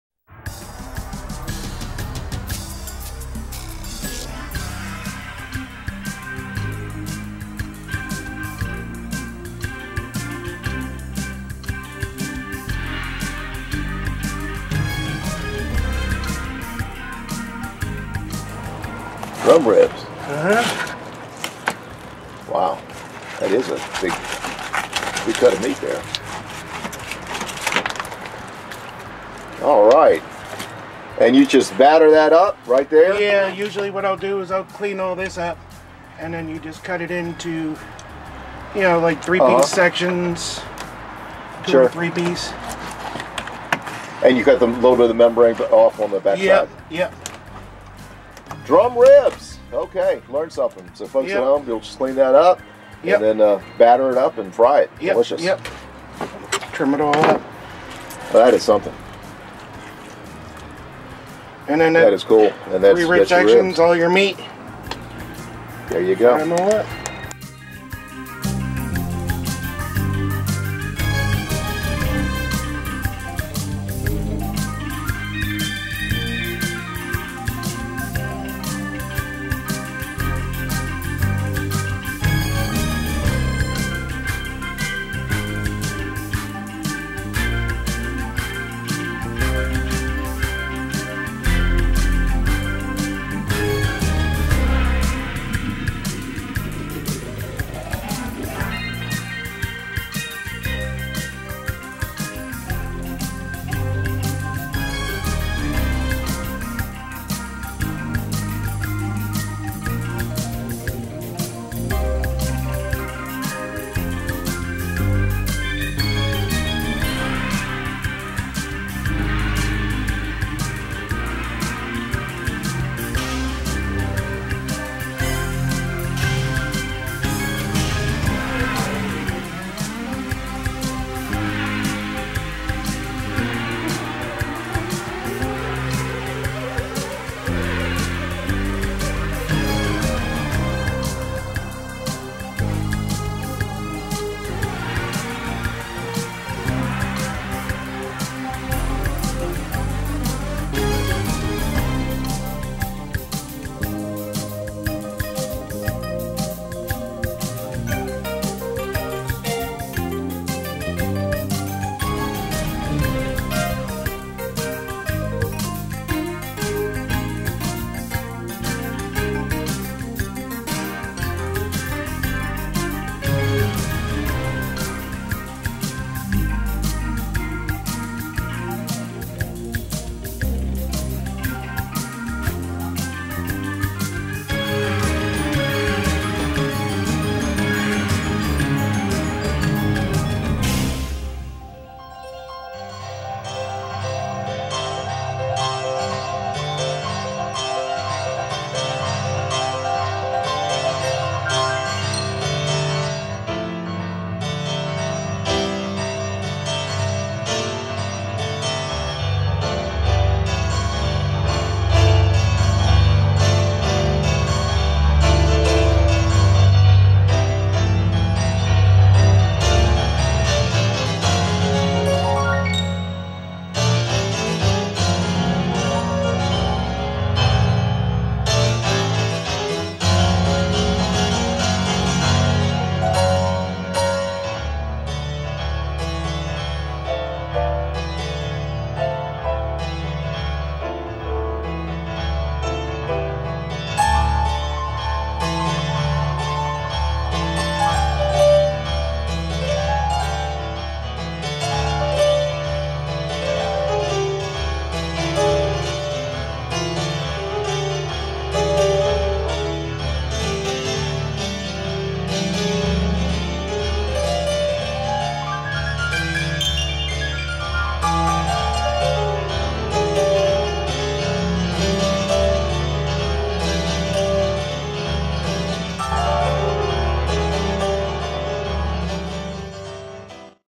How to cook Big Black Drum Ribs & Recipe Eastern Shore Virginia Jim Baugh Outdoors TV
Here is a local favorite on the Eastern Shore featuring black drum ribs. An easy dish to make and once you try it, you will never discard the rib meat again.

Recipe and cleaning tips were given by Mark at Chris' Bait and Tackle Eastern Shore Virginia. Many thanks!

Godspeed,
Jim Baugh JBOTV